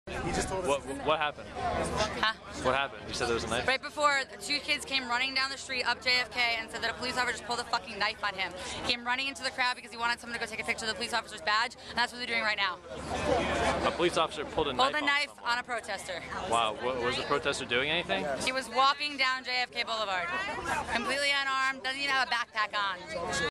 Philly Police Pulled Knife on Someone Night of Occupy Philly Eviction
Over the course of the night police on horseback trampled peaceful protesters standing on the sidewalk after ordering them to move there, sending one to the hospital. Police used bikes as weapons. In two separate instances people said a cop pulled a knife on them. Police choked, beat, and arrested protesters. Protesters remained peaceful. Police got violent. They want the Occupy Wall Street movement to be silent.

We are the 99% and this is only the beginning.

For more media straight from the source at Occupy Philly